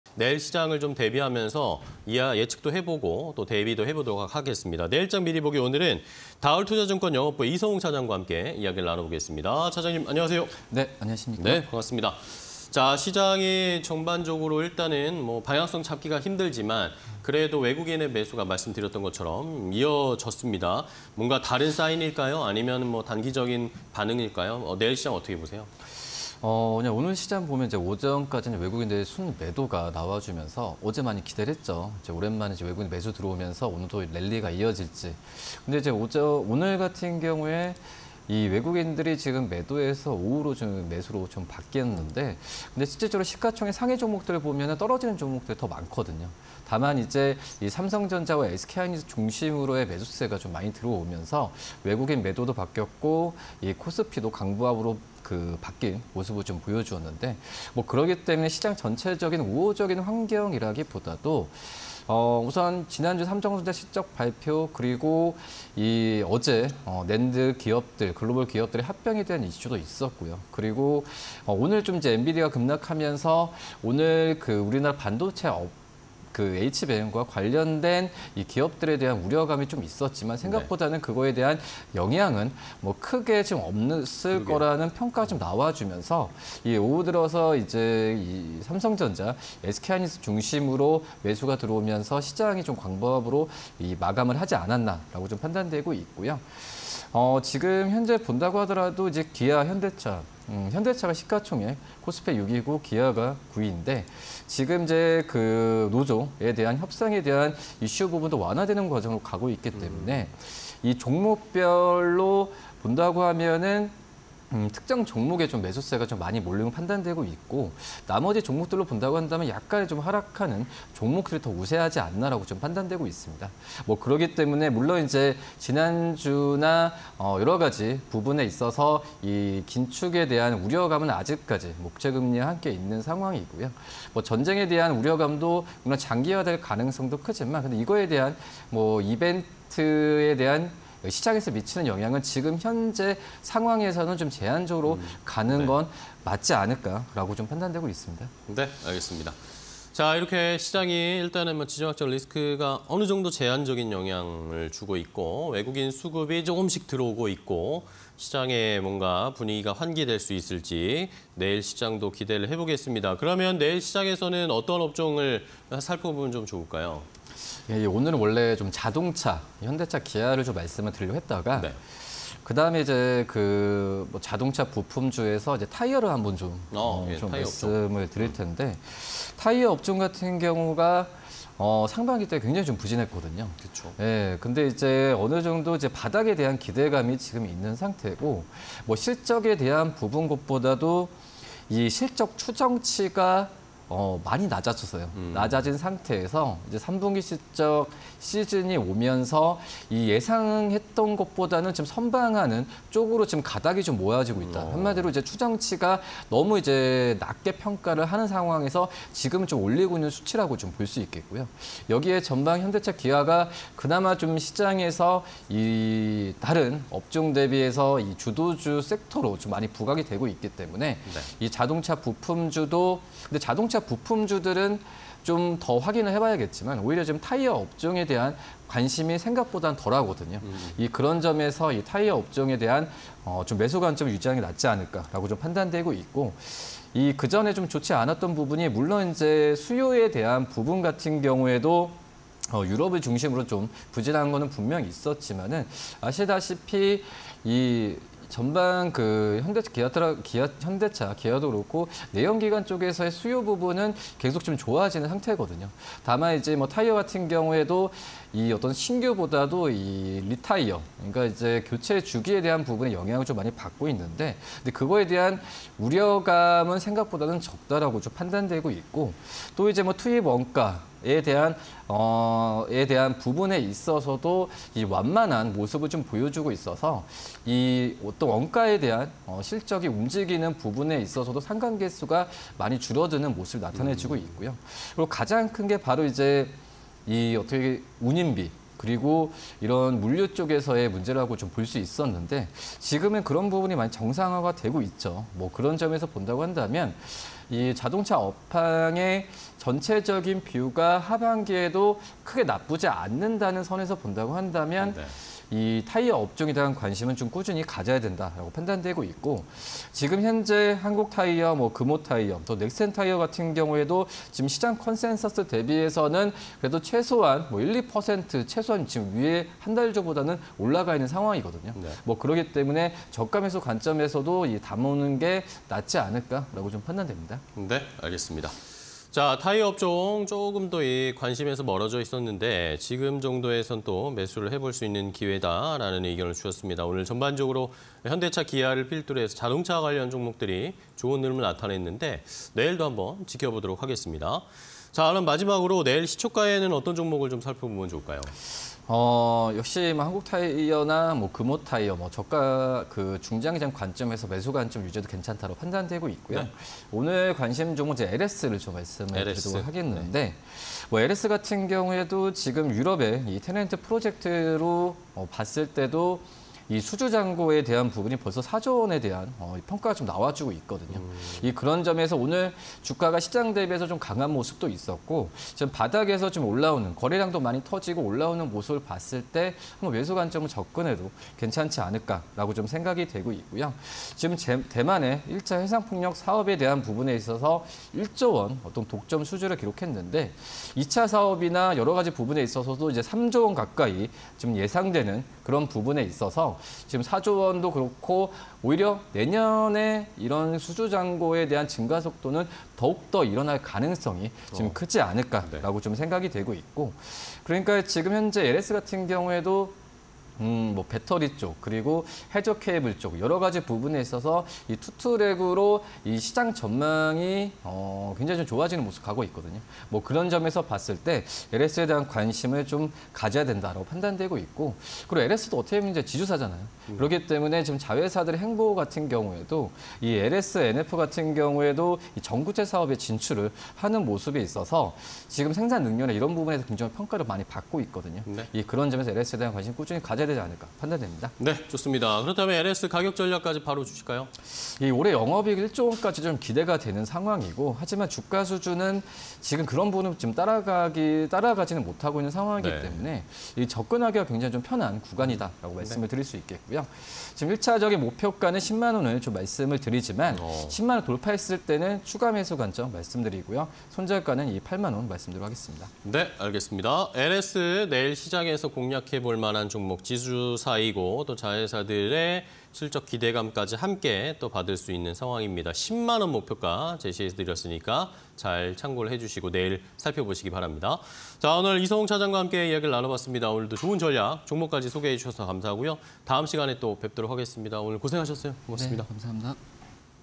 LS(006260)... 해저케이블 수주잔고 4조 원 육박 / 타이어 관련주... 투입 원가 하락 지속_내일장 미리보기 (20231018)
타이어 관련주
- 투입 원가 하락 지속
- 유통망 확장으로 기대감 증가
- 주요 기업들 3분기 실적 ‘예상치 상회’

LS(006260)
- 해저케이블 수주잔고 4조 원 육박
- CAPA 증설 가속화 기대
- 전구체 사업 진출 긍정적